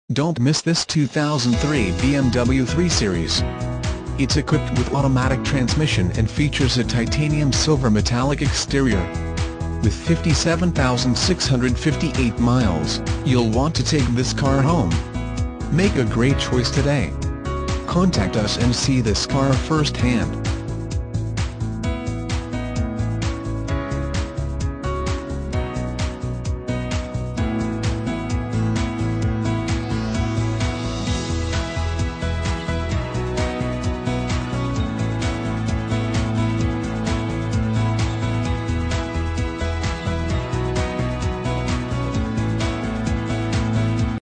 2003 BMW 3-SERIES
2003 BMW 3-SERIES Westmont IL

Don't miss this 2003 BMW 3-Series. It's equipped with automatic transmission and features a Titanium Silver Metallic exterior. With 57658 miles, you'll want to take this car home. Make a great choice today.